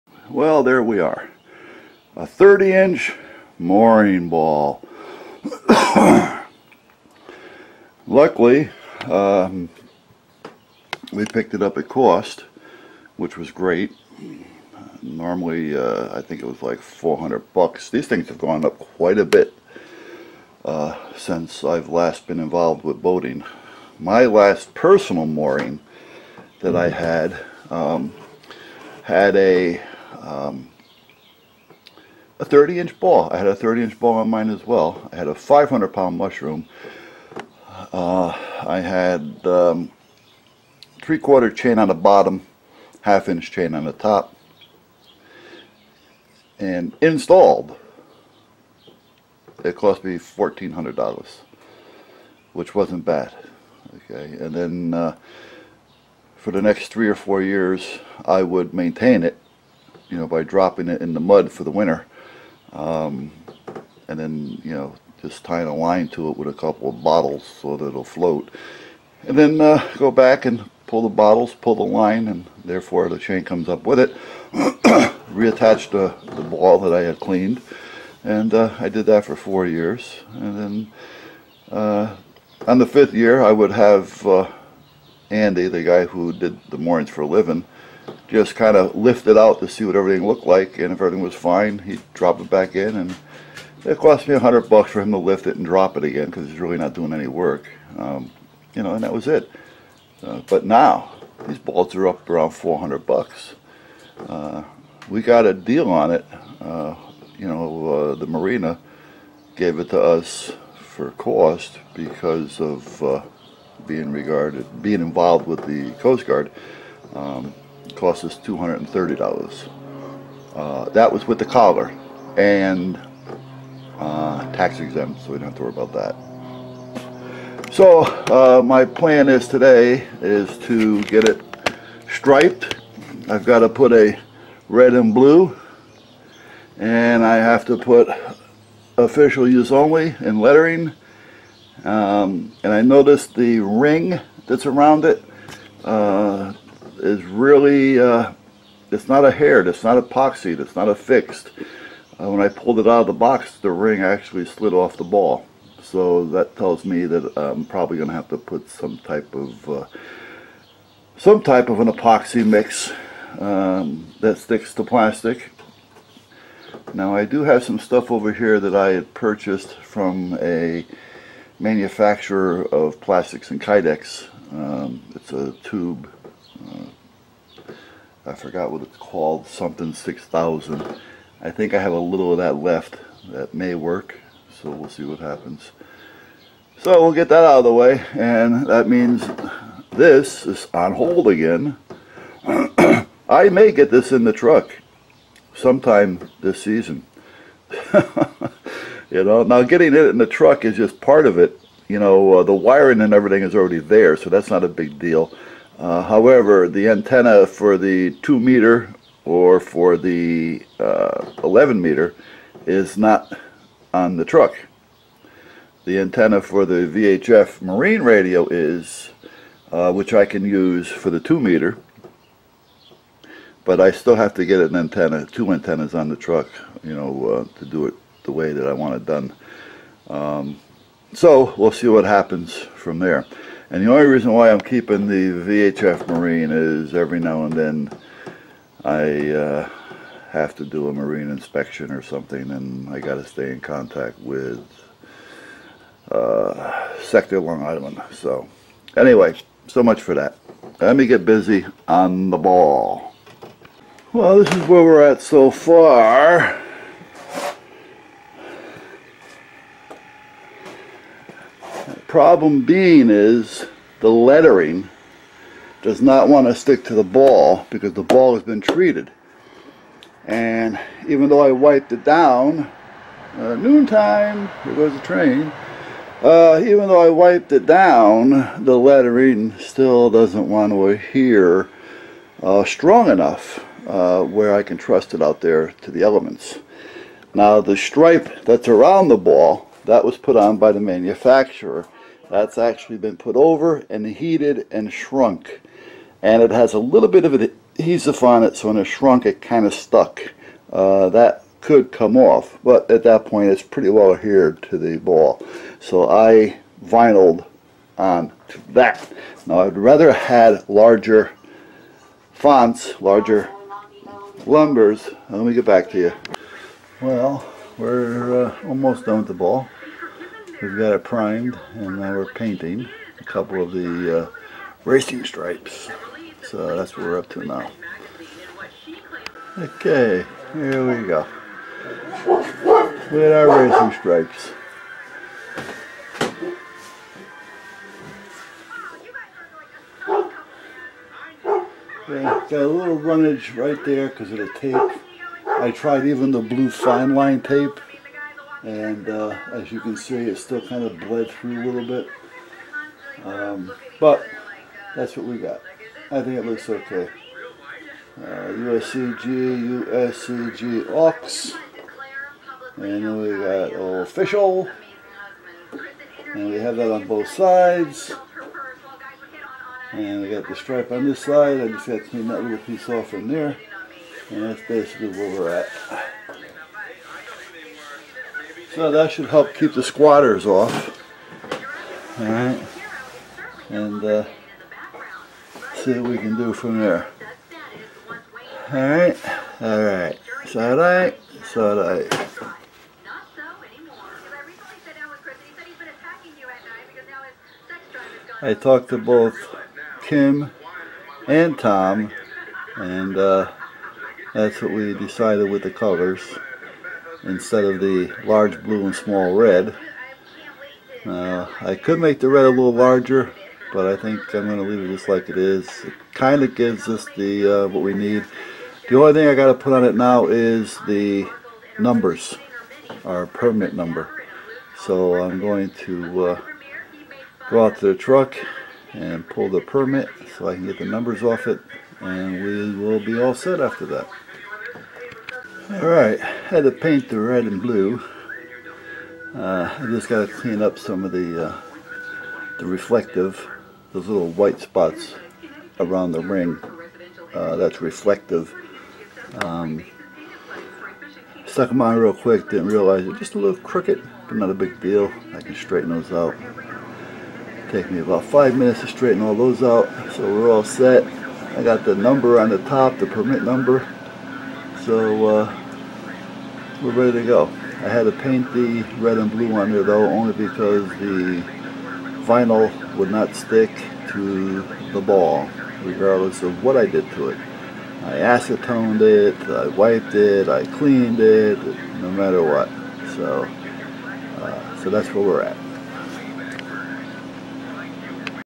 USCG Aux Mooring
Should be done and ready to drop early next week.  30in ball, 1in bottom chain, 1/2in top chain, 300lb mushroom.